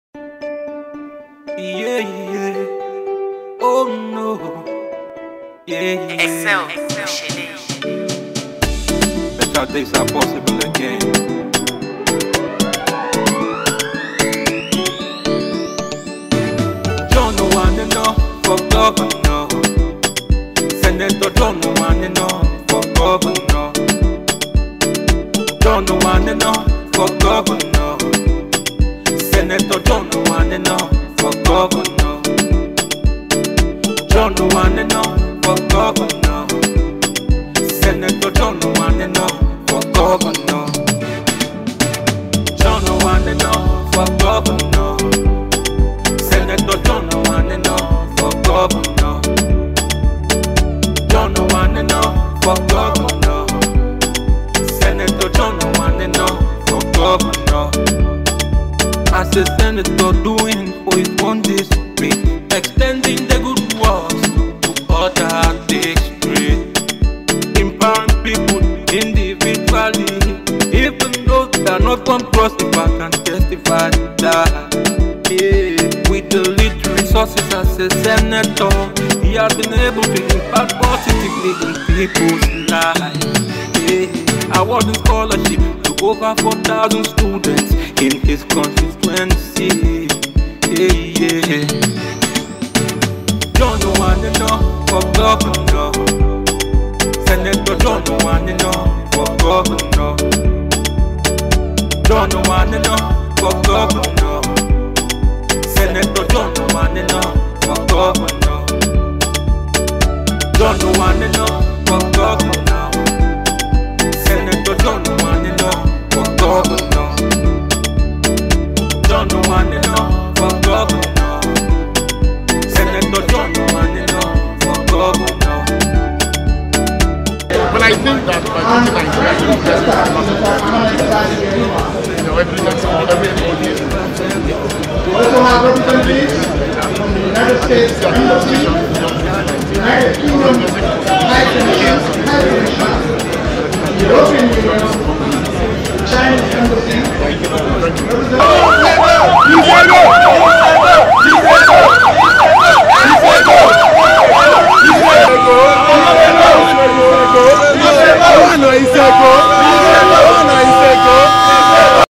APC NATIONAL CONVENTION
APC NATIONAL CONVENTION: SENATOR JOHN OWAN ENOH GOVERNORSHIP CANDIDATE FOR CROSS RIVER STATE